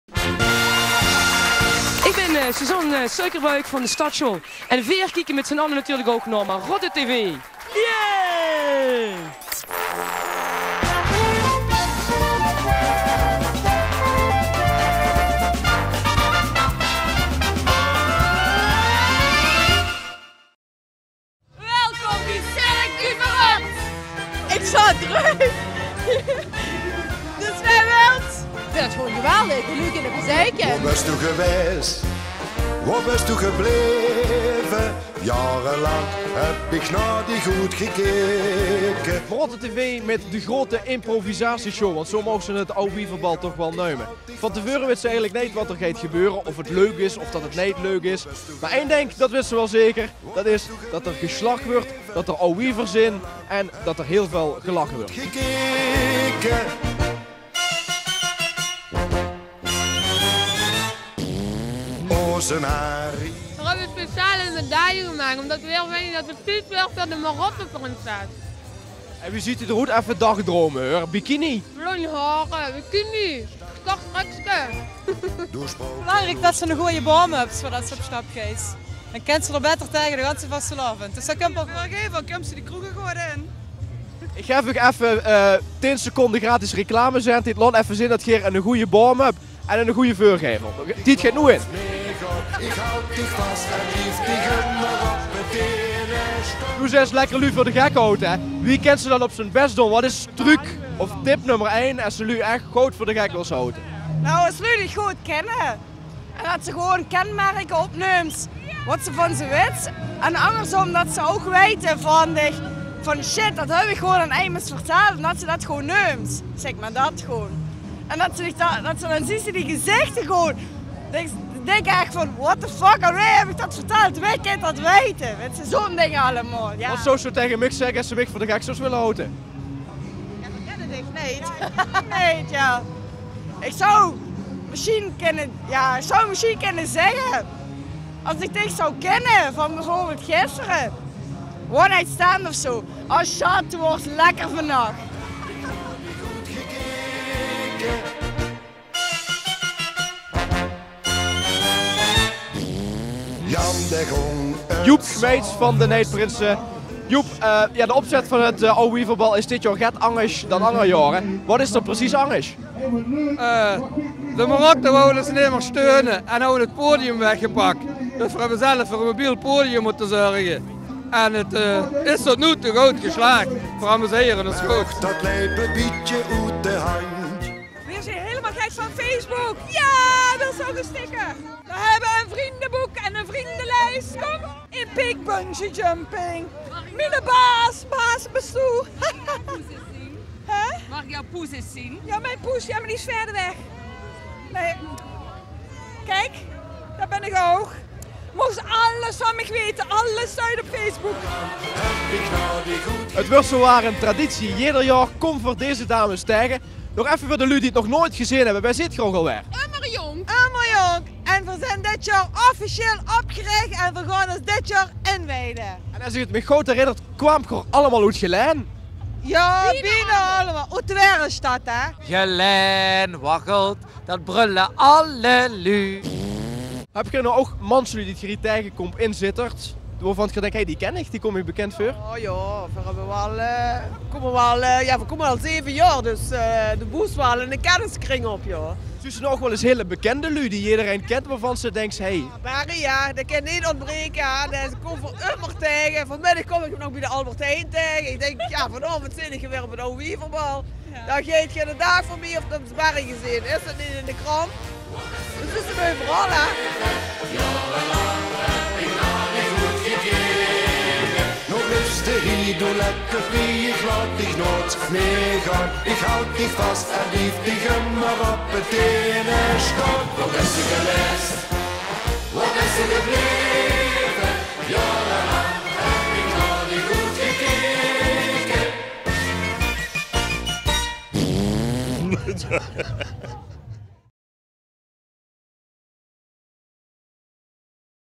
Marotte TV | Nach van 't Auwt Wief 2012
Reportage over de Nach van 't Auwt Wief in de binnenstad van Sittard.